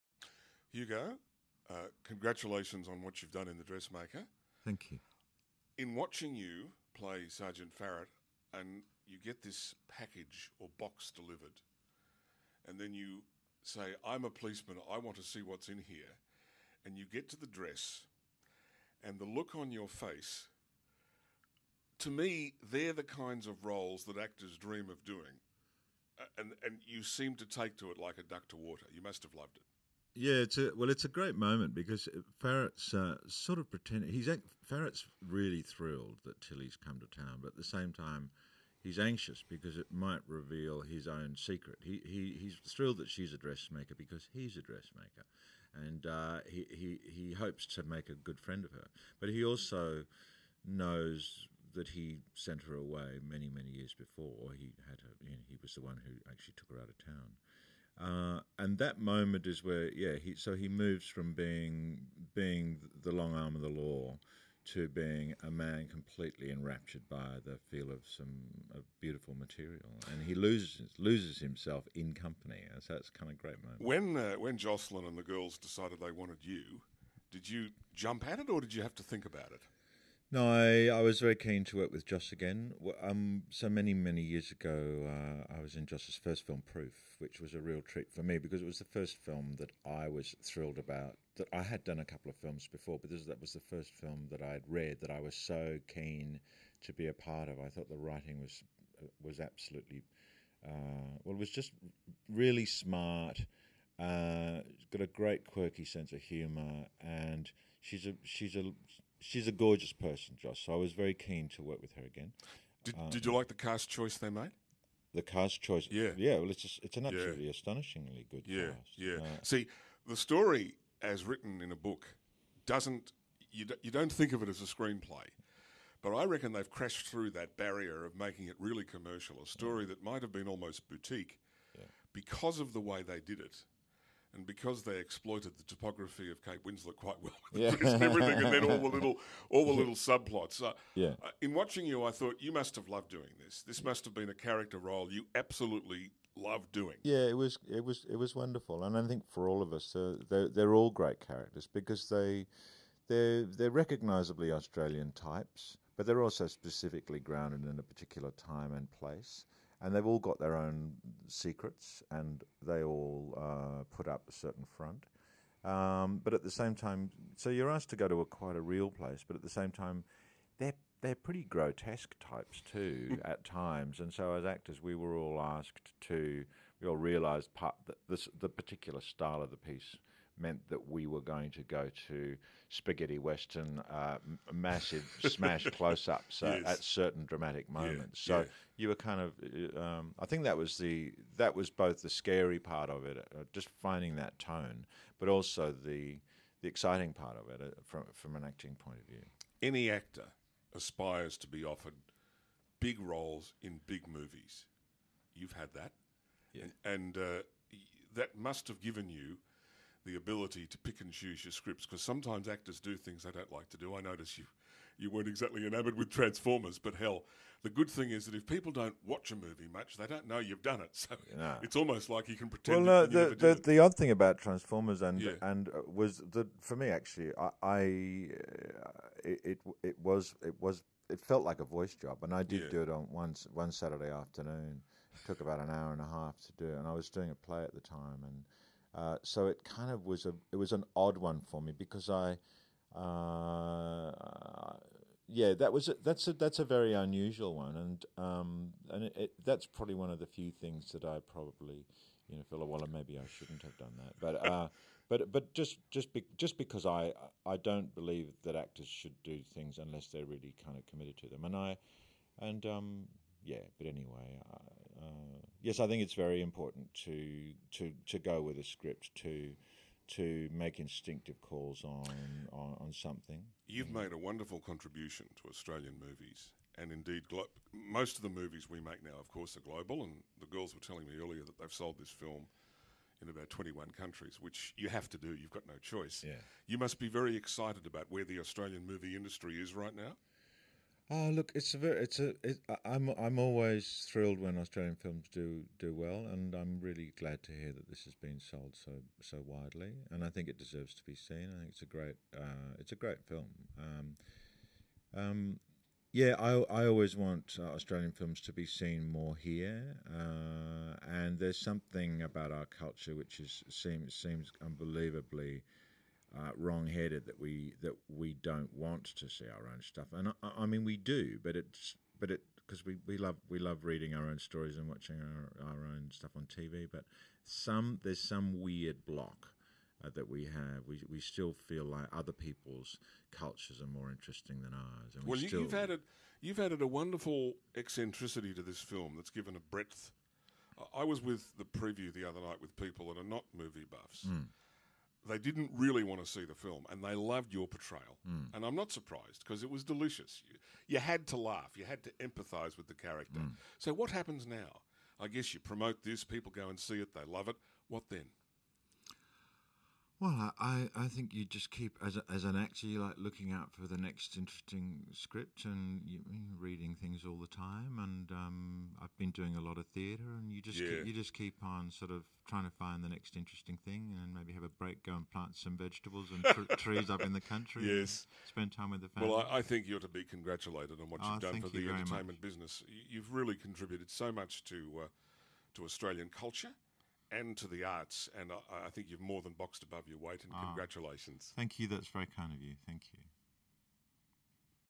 Hugo Weaving – The Dressmaker
Leon Byner chats with Australian actor Hugo Weaving about his role in the new film The Dressmaker.